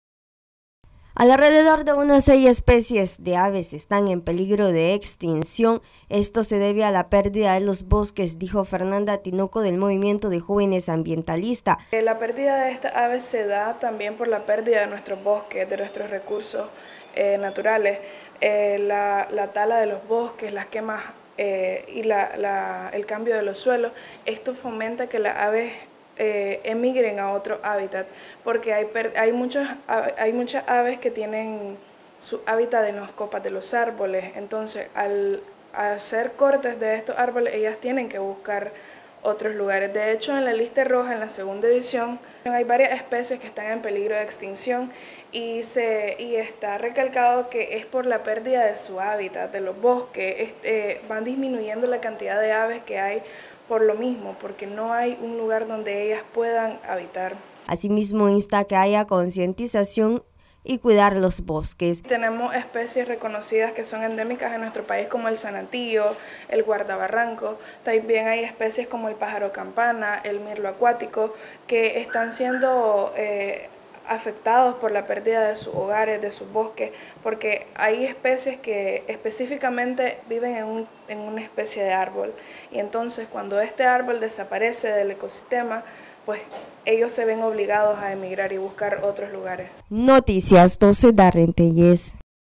Más de 6 especies de aves se encuentran en peligro de extinción
Noticias12Nicaragua Movimiento de Jóvenes ambientalistas ve con mucha preocupación que algunas aves estén en peligro de extinción.